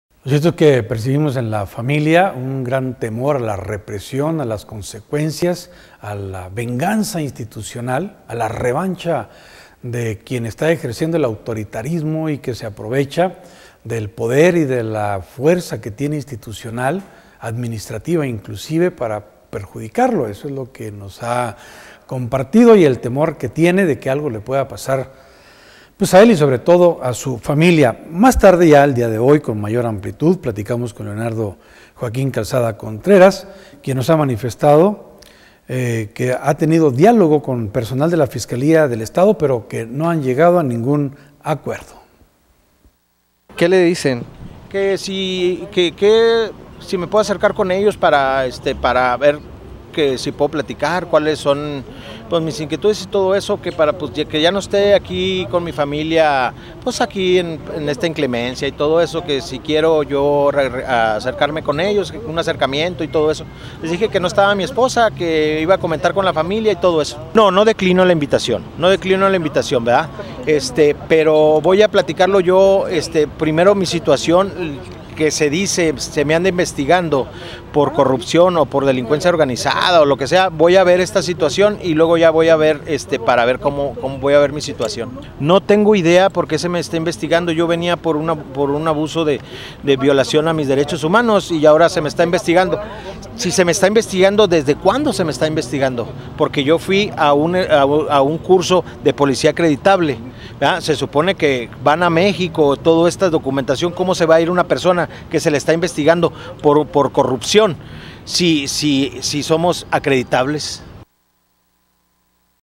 Policía manifestante dialogo esta mañana con personal de la fiscalía general
Leonardo Joaquín, Policía manifestante dialogo esta mañana con personal de la fiscalía general del estado pero no llegaron a un acuerdo seguirá en la plaza Hidalgo